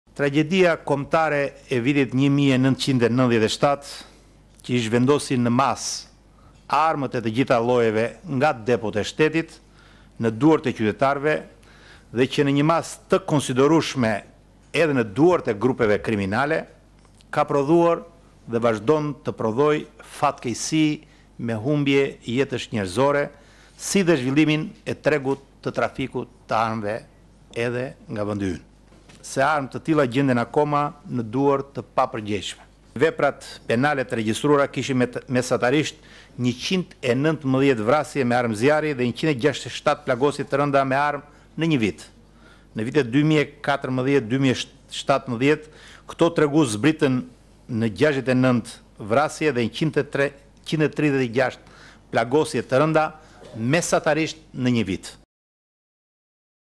Ora News - Xhafaj: Armët në duart e të papërgjegjshmëve, në 2019 aksion
Ministri i Brendshëm, Fatmir Xhafaj, deklaroi sot se armët janë ende një problem i prekshëm për Shqipërinë dhe se qeveria do të angazhohet për shkatërrimin e plotë të armëve të tepërta duke nisur nga viti i ardhshëm.

Duke folur në një takim të zhvilluar sot në kuadër të luftës kundër trafikimit të armëve, Xhafaj tha se puna për krijimin e një strategjie dhe planiveprimi ka nisur dhe do të prezantohen në 2019 në Paris.

Xhafaj: Keqpërdorimi i armëve dhe mandej trafikimi i paligjshëm i tyre përbëjnë një shqetësim vërtet serioz. Shfrytëzimi i tyre nga organizatat kriminale dhe më tej nga celulat terroriste ,mbetet problematik.Shqetësimi kalon kufijtë rajonalë.Është ngritur një komision kombëtar për të kordinuar dhe monitoruar të gjitha aktivitetet në vend që kanë lidhje me armët e vogla dhe të lehta. Komisioni kombëtar dhe ekspertët po punojnë për të hartuar një strategji dhe plan veprimi që do prezantohen në takimin e dhjetorit në Paris.

Xhafaj theksoi se armët janë në duart e njerëzve të papërgjegjshëm.

Sipas Xhafajt, viti 1997 ka lënë pasojat e veta me humbje njerëzore pas hapjes së depos së armëve dhe  se armët janë akoma në duart e njerëzve të papërgjegjshëm.

Xhafaj: Jemi të ndërgjegjshëm se armë të tilla gjenden akoma në duar të njerëzve të papërgjegjshëm. Tragjedia kombëtare e vitit 1997 që zhvendosi në masë armë nga depot në duart e qytetarëve ka prodhuar e vazhdon të prodhojë fatkeqësi me humbje jetësh njerëzore. Është dashur një punë e jashtëzakonshme në vitet për të zbutur efektet e asaj çmendurie. Shqipëria ka arritur rezultate të mira në luftën kundër trafikimit të armëve.

Janë ndëshkuar rastet e armëmbajtjeve pa leje, kjo ka çuar në ulje të ndjeshme të vrasjeve me armë zjarri. Këto shifra janë shumë më të ulëta edhe në këtë vit që jemi. Por përsëri me mesetare të ulët në krahasim me standartet e BE. Fushata ndërgjegjësuese për dorëzimin e armëve ka rezultuar përgjithësisht pozivite. Ministria e Brendshmme është e angazhuar që në vitin 2019 të nisë operacionin për shkatërrimin e plotë të armëve të tepërta. Reformat e vëna në lëvizje kanë pikërisht këtë qëllim.

Xhafaj tha se policia e shtetit në bashkëpunim me agjencitë ligjzbatuese është përfshirë në operacion të gjerë në kontrollin e  territorit për identifikimin, goditjen dhe ndëshkimin e grupeve kriminale që janë përfshirë në përdorimin dhe trafikimin e armëve të zjarrit të të gjitha llojeve.

Sipas Ministrit të Brendshëm, megjithëse legjislacioni penal është ndër më të ashprit në rajon, ekziston një problematikë reale përsa i përket armëmbajtjes pa leje dhe trafikimit të armëve.

Xhafaj: Një kuadër ligjor më i qartë dhe kryesorja implementimi i duhur do të ndihmojë më mirë në parandalimin e ngjarjeve të kronikës së zezë në këtë fushë.

Takimi i sotëm ishte organizuar nga BE dhe lançuar nga Gjermania dhe Franca.